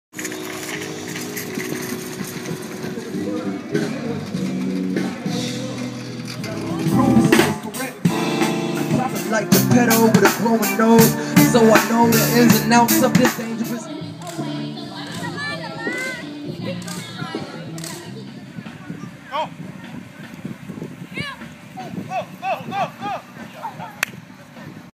Bicycle Block Party
The Bicycle Block Party, part of the Play Streets series, was held May 19 at Savannah Gardens.